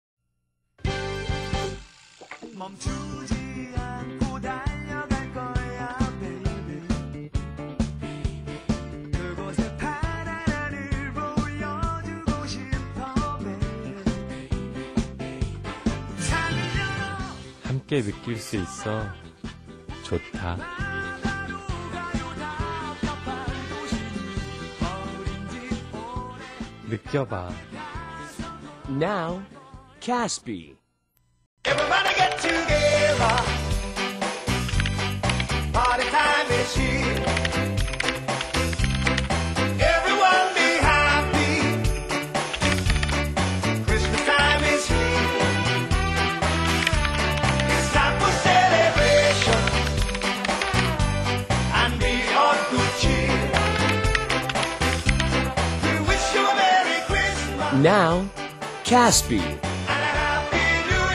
Kim Tae Hee's Caspi CF (old video of 2001)
Kim Tae Hee's Caspi CF of 2001
Tae Hee's 12 years ago video!